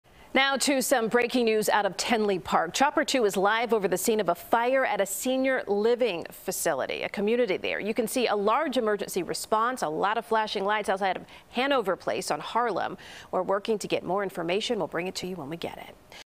Fire reported at Tinley Park senior living community
The view from Chopper 2 showed a large emergency response, but no smoke or fire.